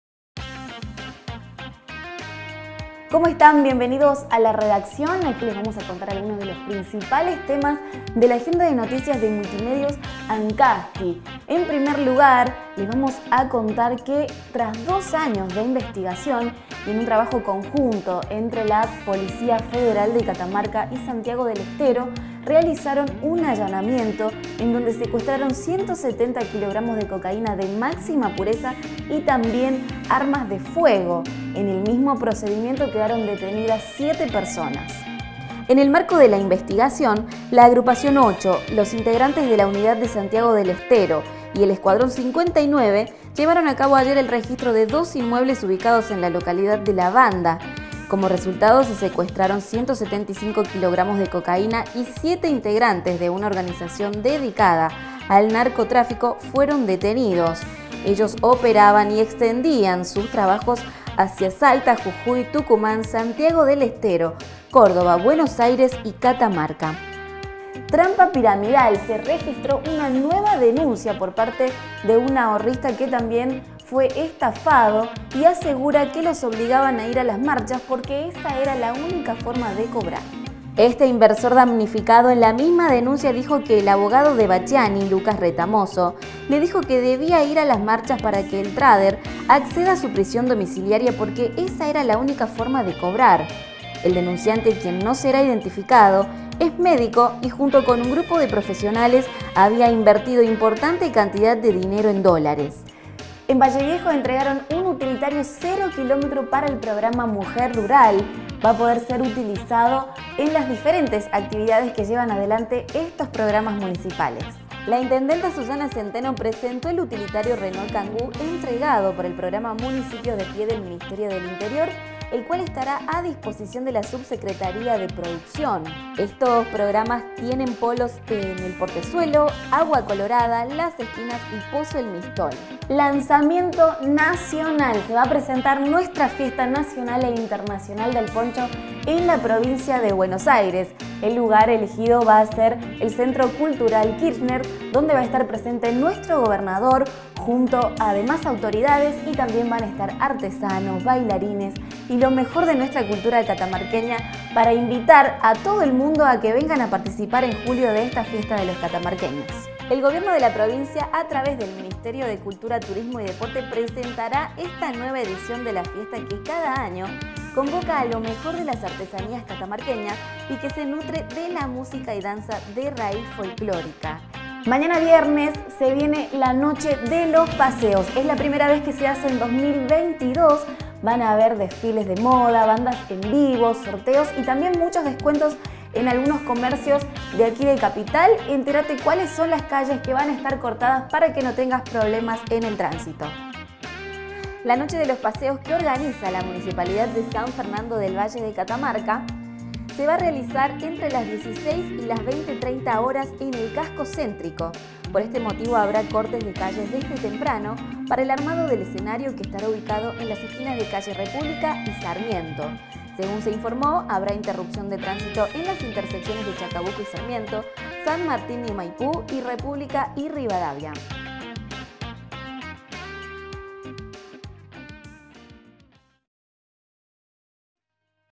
La Redacción: Este viernes se llevará a cabo la noche de los paseos
Este viernes se realizará una nueva edición de “La Noche de Los Paseos ”, que organiza la municipalidad de San Fernando del Valle de Catamarca.
El evento se realizará entre las 16 y 20.30 horas, en el casco céntrico. Por este motivo, habrá cortes de calle desde temprano para el armado del escenario, que estará ubicado en la esquina de calles República y Sarmiento.